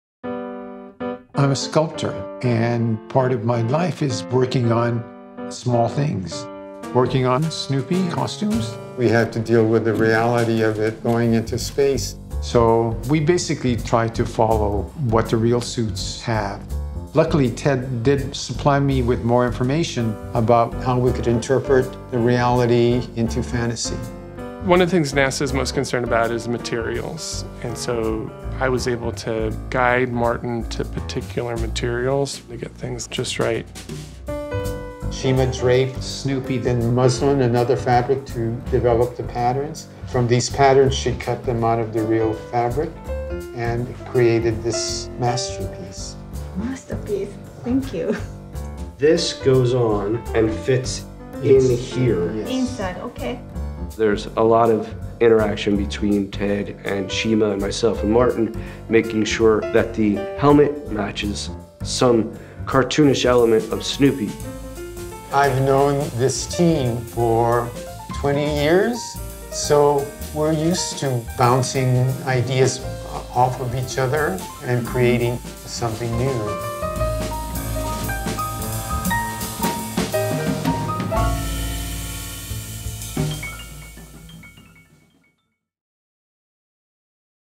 Suiting Up Snoopy: The Making of Snoopy for Artemis I
Discover the teamwork behind making Snoopy and his space suit for Artemis I.